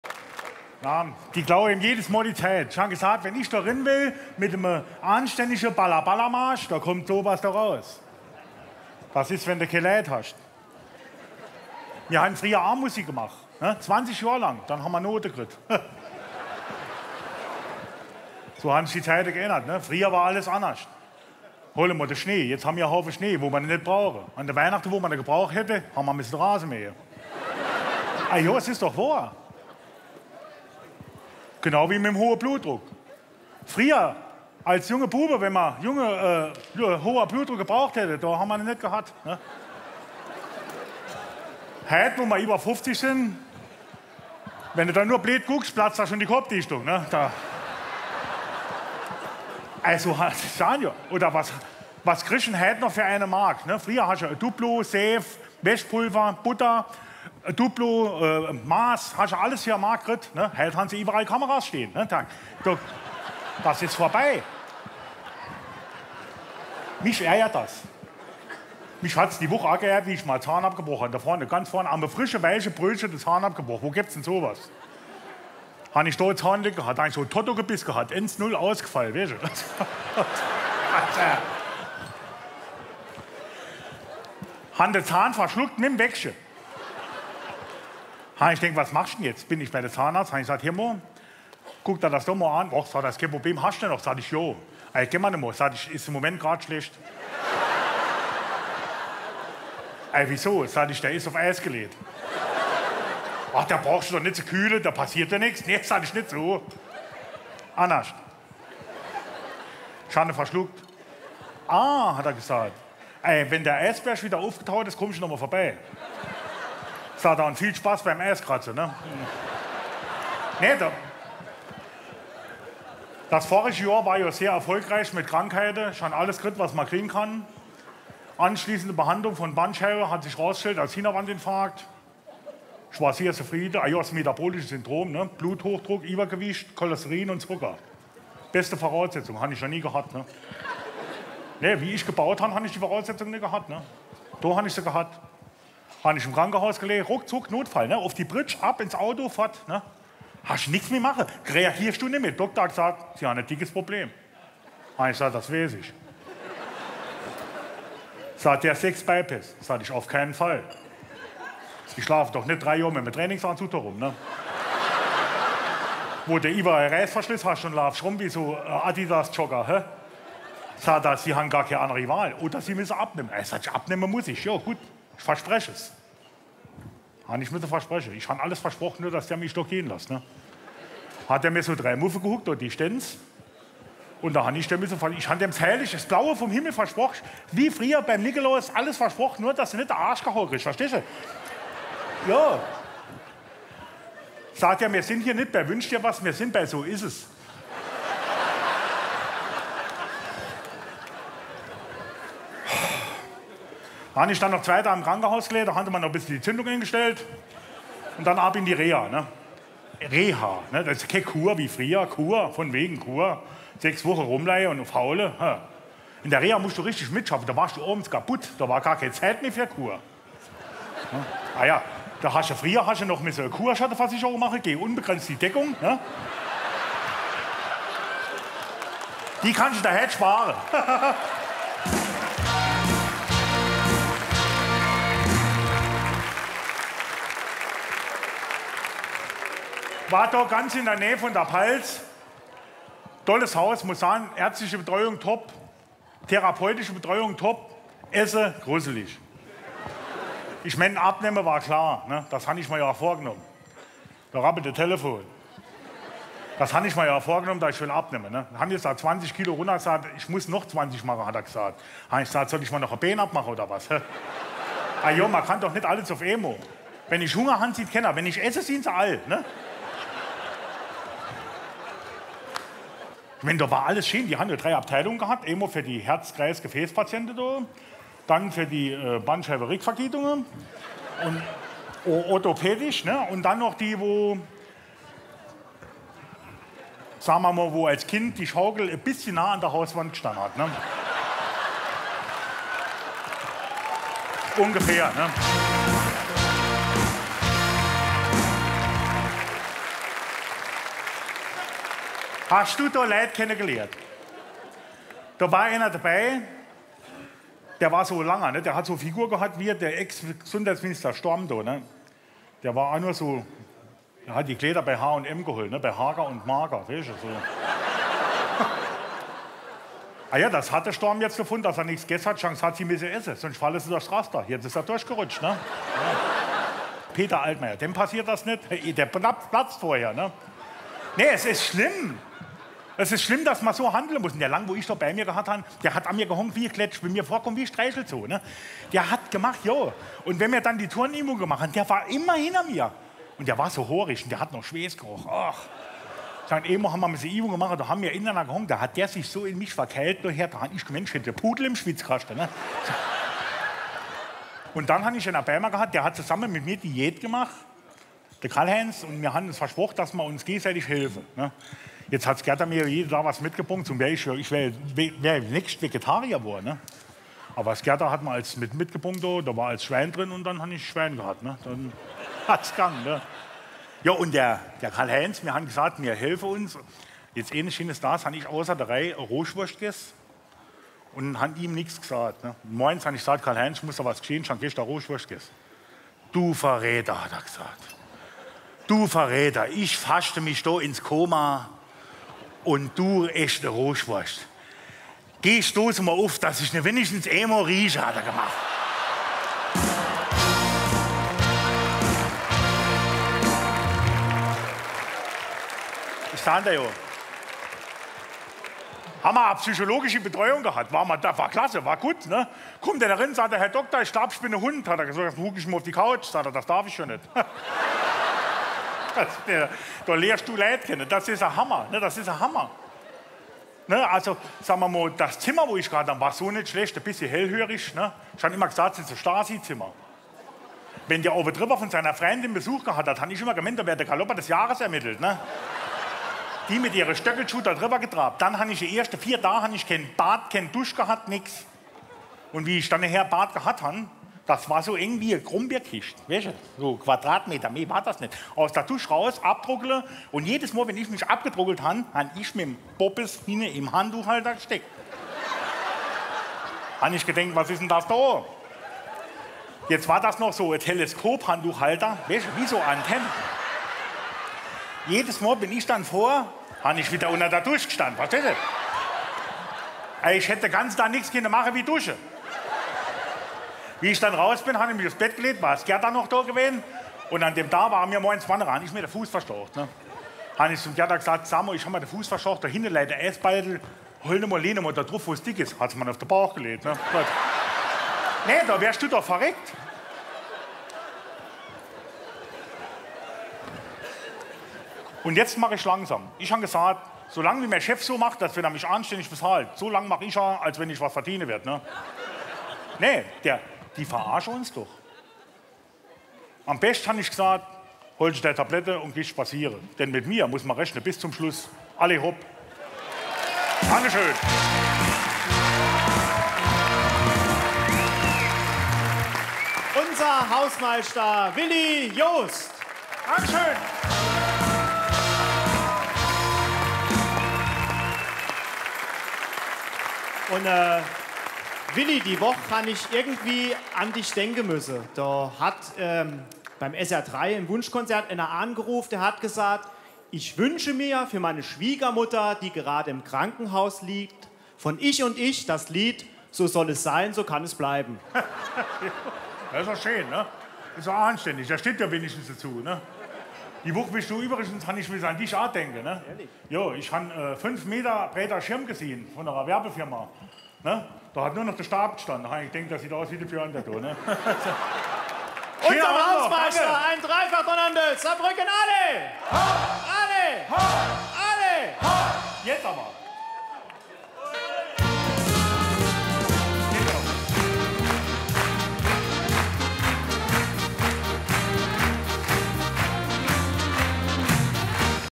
Willi Jost als De Hausmeischda bei Alleh hopp Saarbrücken 2015
Auftritt bei der Prunksitzung der Grossen Saarbrücker Karnevalsgesellschaft „M'r sin nit so"